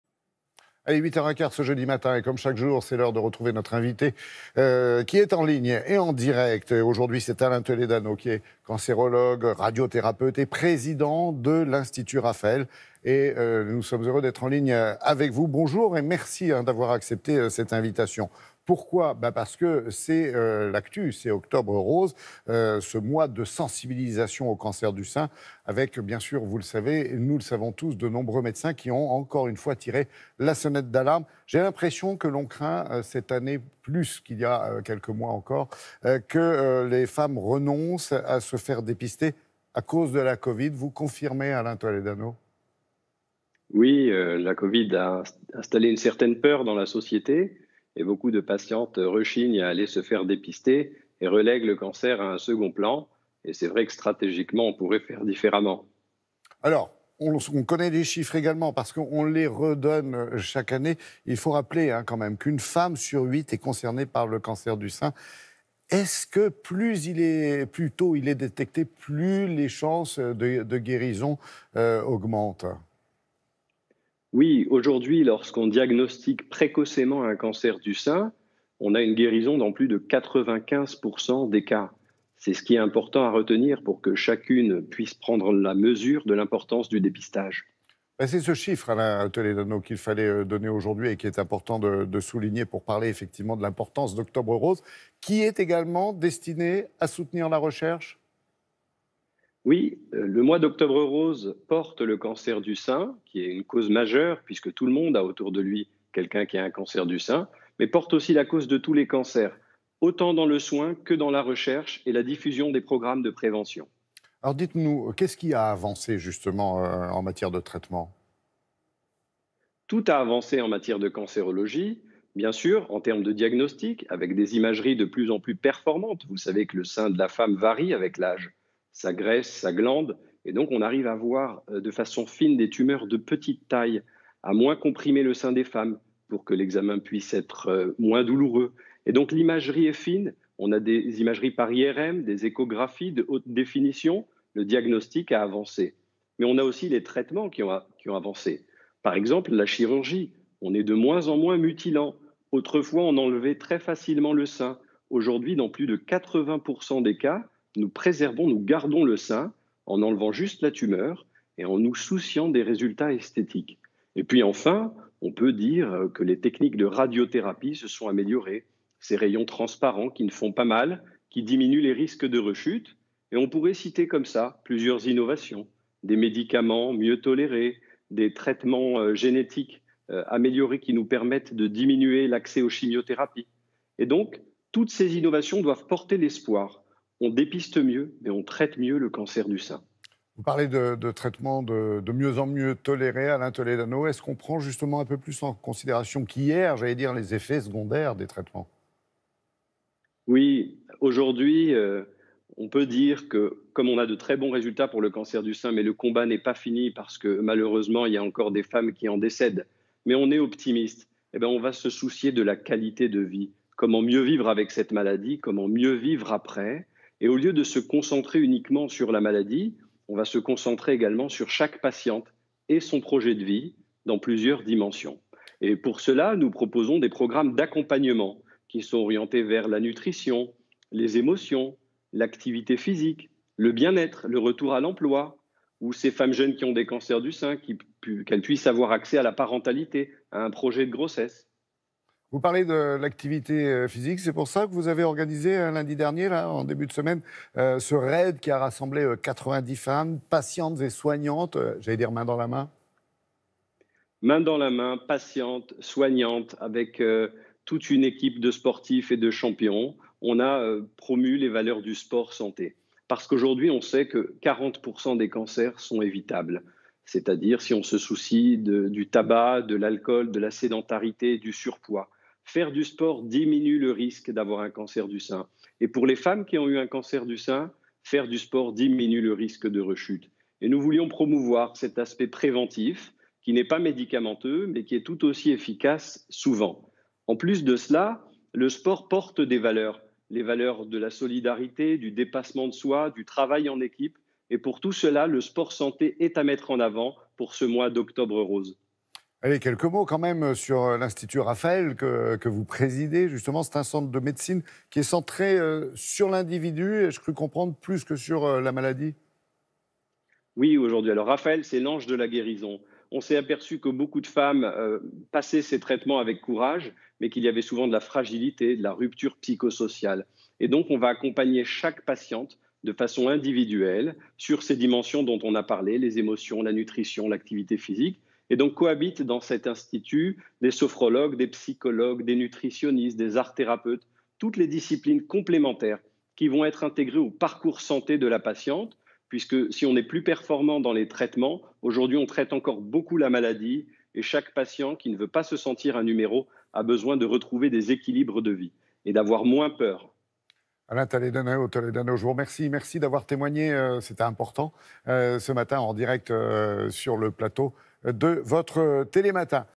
L'invité du jour : Docteur Alain Toledano
Le cancer du sein touche chaque année 59 000 femmes en France. La recherche a fait d’énormes progrès ces 30 dernières années et a permis de mettre au point des traitements toujours plus performants tout en préservant la qualité de vie des femmes. Malgré tout, avec 12 000 décès chaque année, il reste la première cause de mortalité par cancer chez les femmes.
Le docteur Alain Toledano est notre invité du jour...